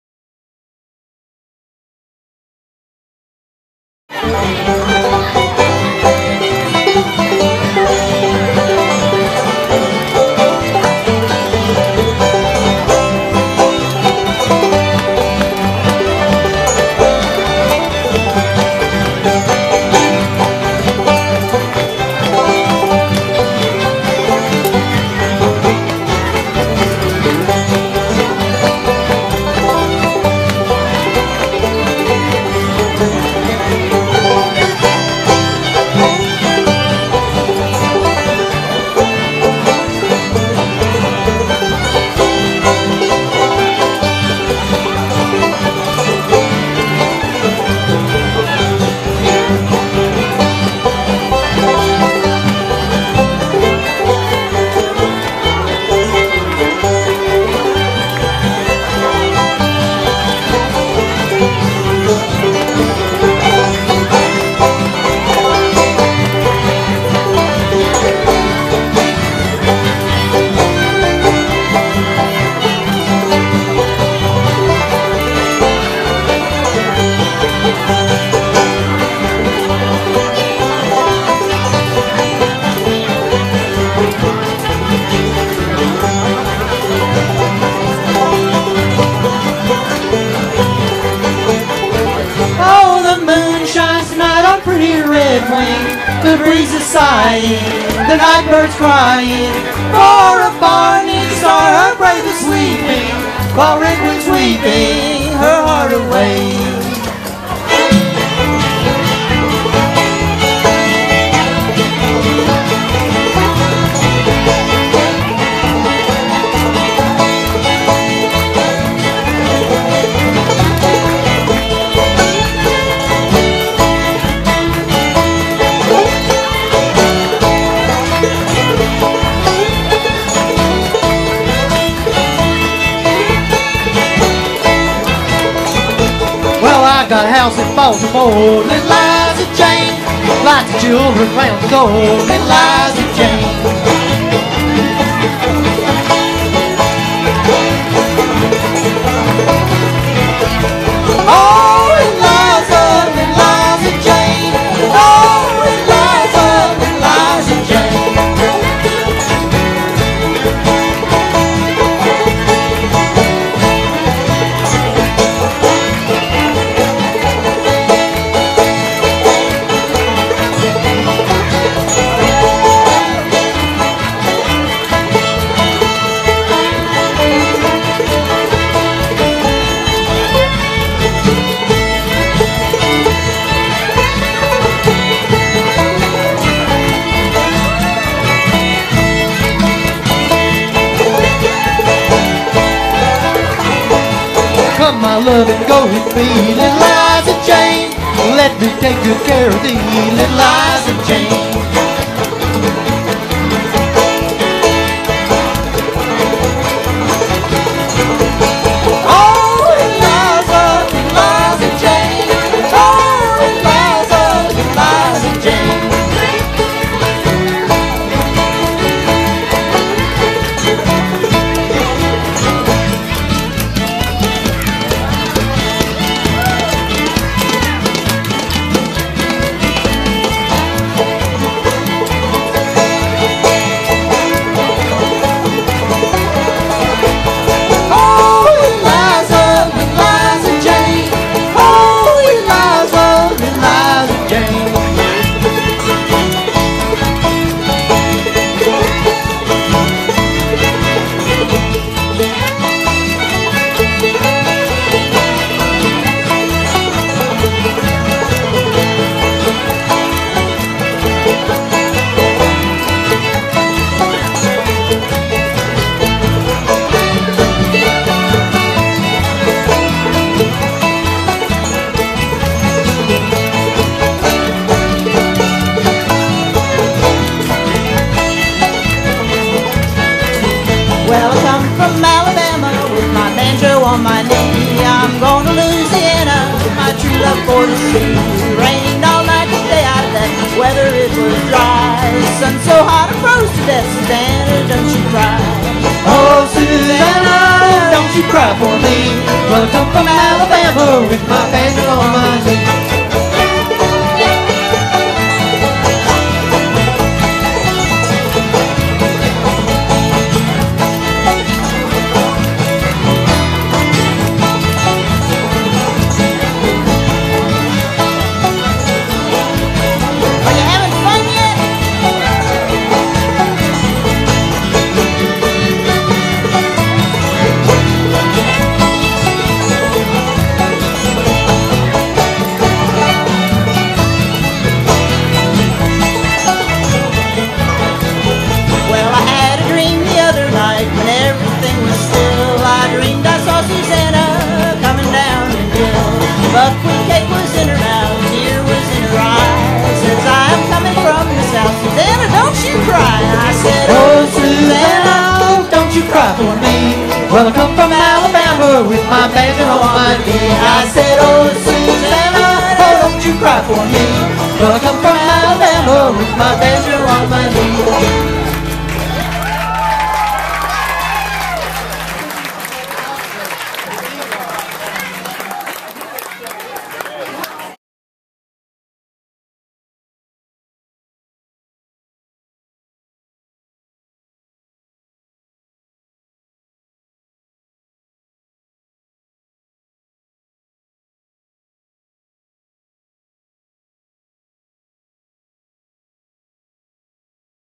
The McKenzies & Friends at the Floyd Country Store
Friday nights at the Floyd Country Store in Floyd, VA are for dancing!
This video was taken using a handheld Flip camera (thanks, Wendy!)
with more sound added from a direct recording off the pa head (thanks,  Skip!).   Out on the dance floor, the sound of the dancers' metal taps is about as loud as the music! The songs in this medley are familiar  songs that have also become fiddle tunes-- fun to sing, play and dance to. The Floyd Country Store is a real piece of Americana. Check it out online and then go dance some yourself along the Crooked Road: